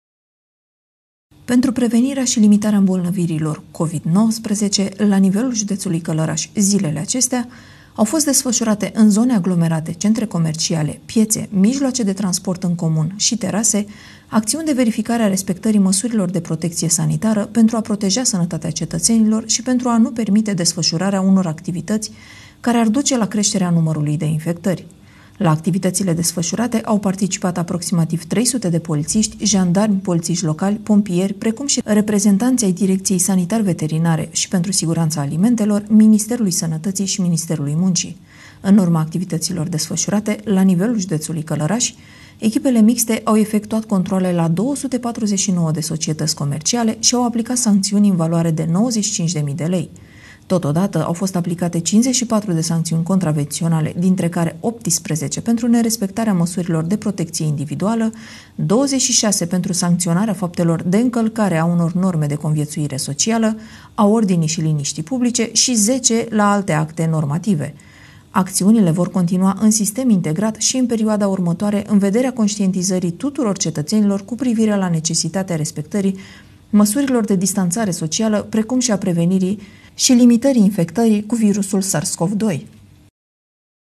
Controale la 249 de societăți comerciale
În urma activităților desfășurate, la nivelul județului Călărași, echipele mixte au efectuat controale la 249 de societăți comerciale și au aplicat sancțiuni în valoare de peste 95.000 lei.